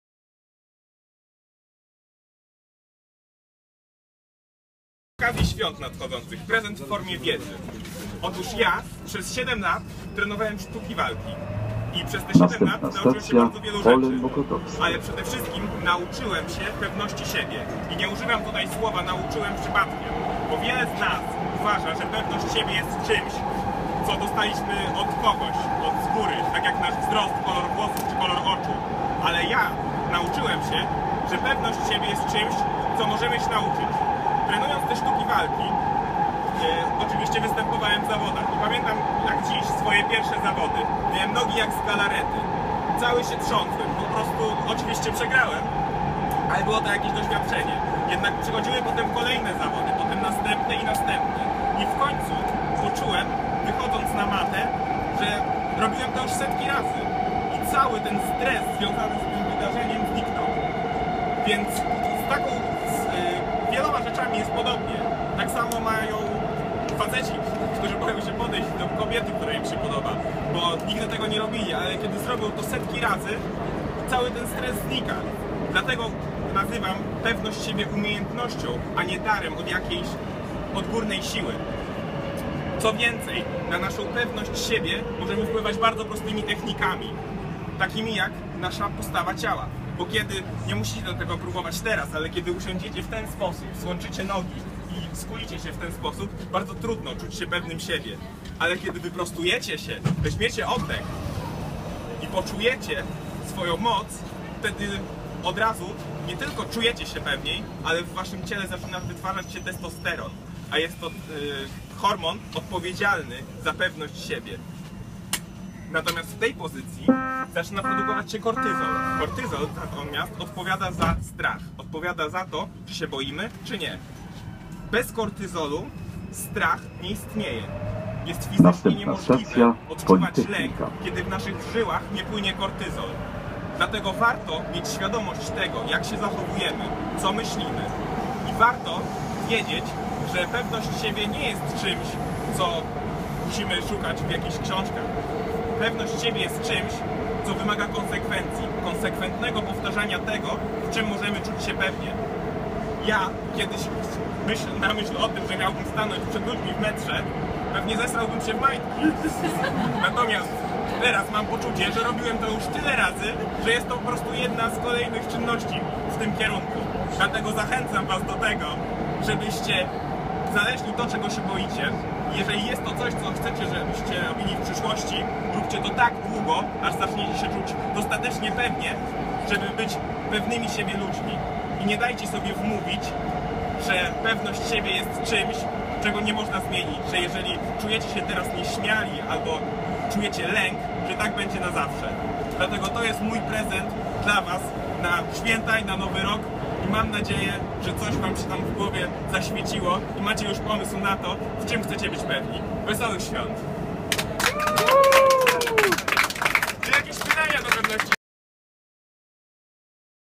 Antoni Łącki prowadzi wykład w warszawskim metrze.
Antoni Łącki przeprowadza w Warszawskim metrze krótki wykład na temat pewności siebie.